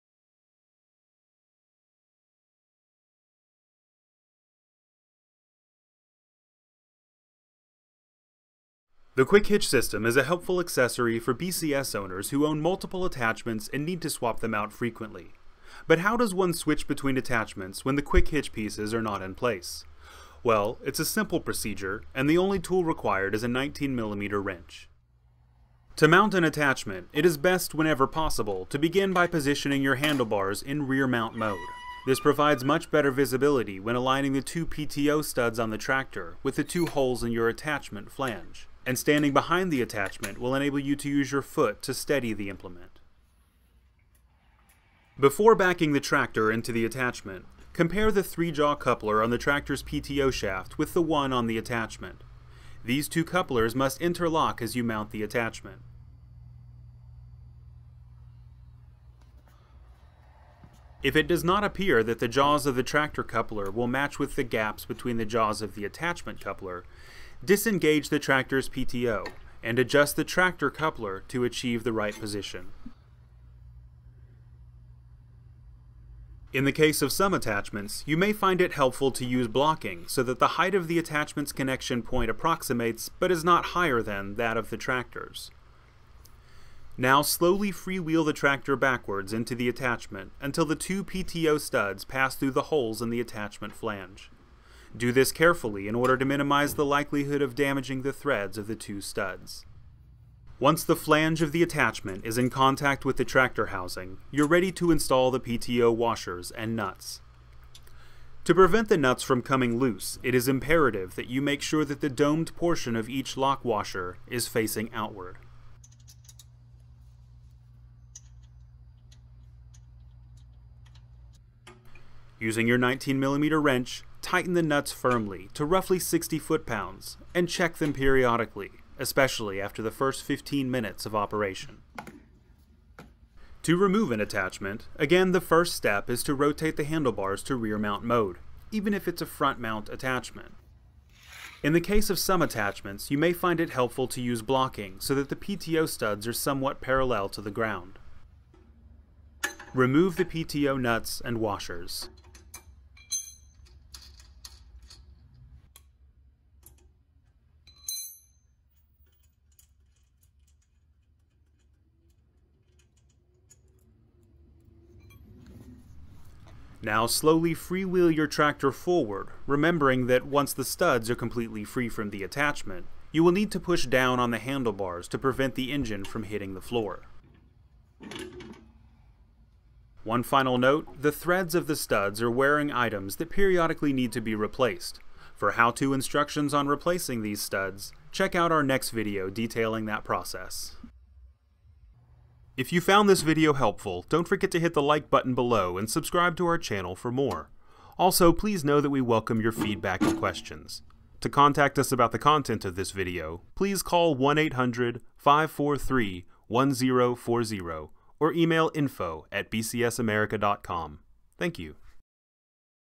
Swapping Attachments on a BCS Tractor Without a Quick Hitch
This video will teach you how to attach and detach attachments directly onto the PTO studs of a BCS Two-Wheel Tractor. For a faster way to switch between attachments, check out our Quick Hitch system

If you need service or replacement parts, find your local BCS dealer